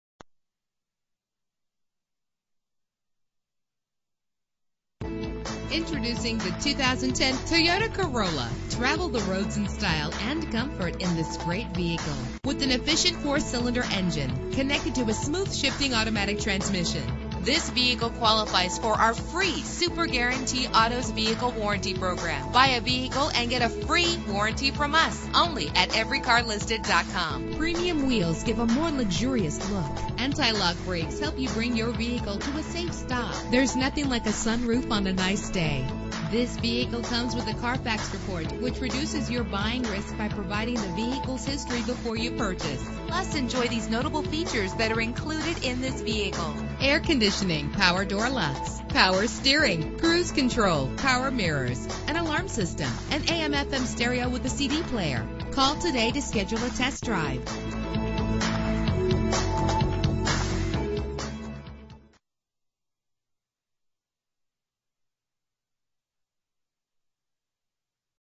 2010 Toyota Corolla - Jacksonville FL
Year: 2010
Make: Toyota
Model: Corolla
Trim: S
Engine: 1.8 liter inline 4 cylinder 16 valve
Transmission: 4-Speed Automatic
Color: Dark Gray
Mileage: 20147
Address:
10939 Atlantic Bl